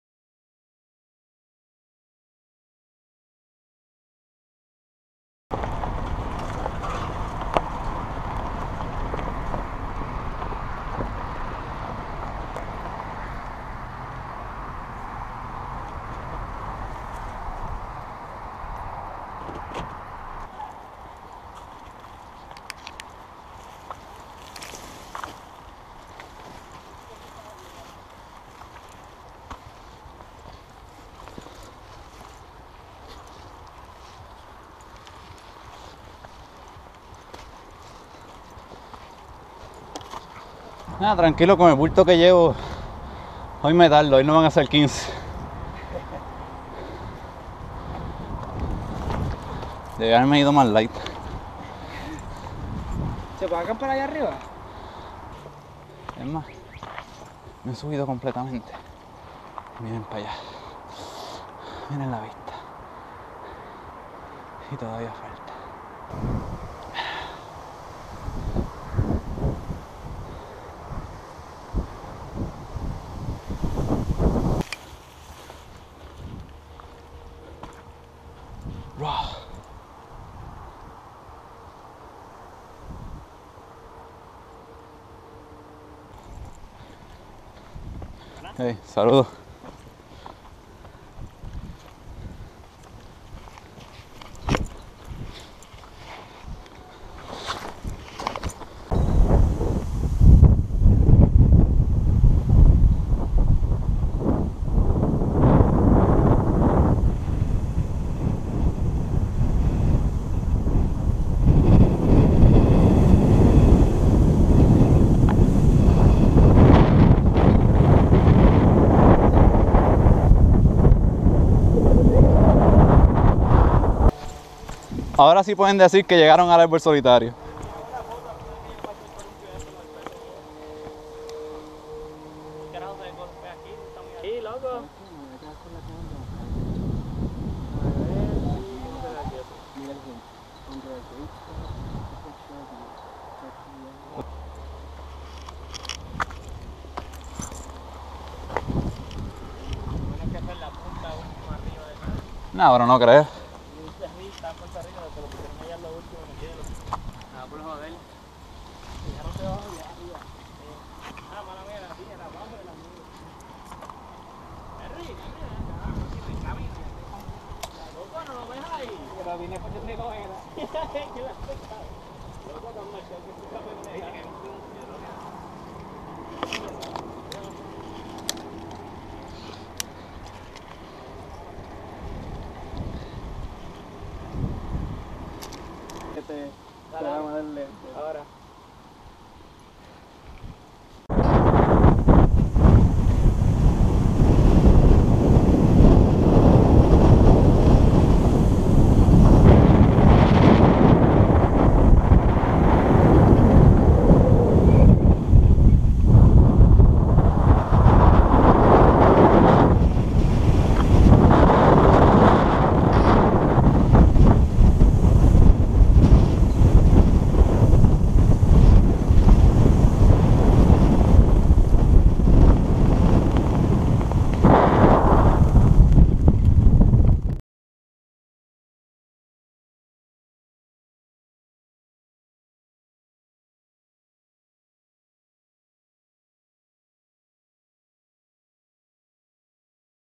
La travesia hacia el arbol solitario
La travesia el 24 de diciembre de camino a acampar en uno de los spots con mejor vista en la isla. El arbol solitario en Cayey.